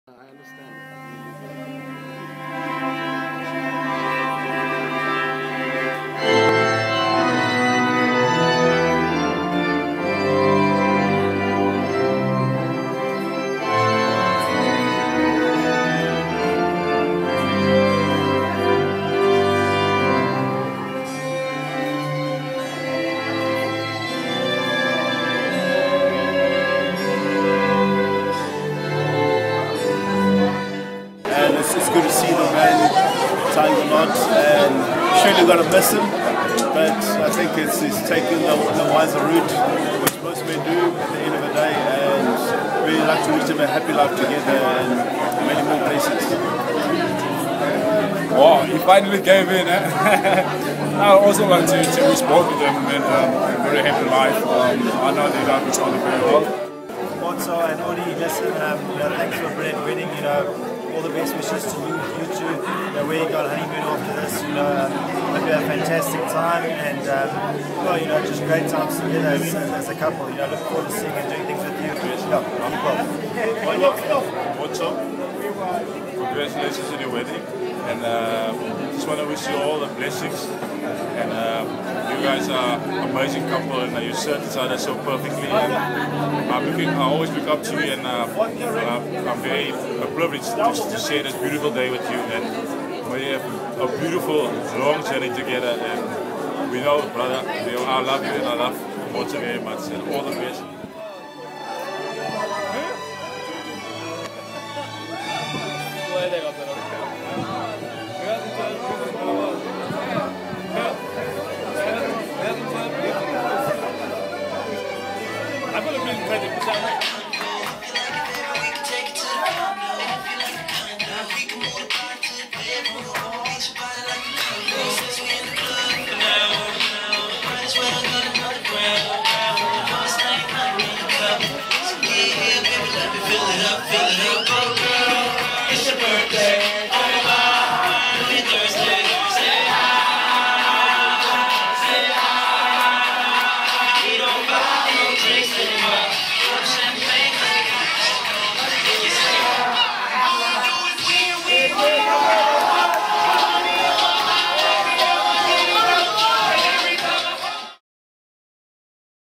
Odwa and Kgomotso's Wedding
The new Mrs. Ndungane, Kgomotso, chose to have her white wedding with a vintage romance theme at the castle and chapel of Camelot at Greensleeves in Hillcrest, Durban. On the rainy day, their 240 guests were treated to a wonderland that was their reception.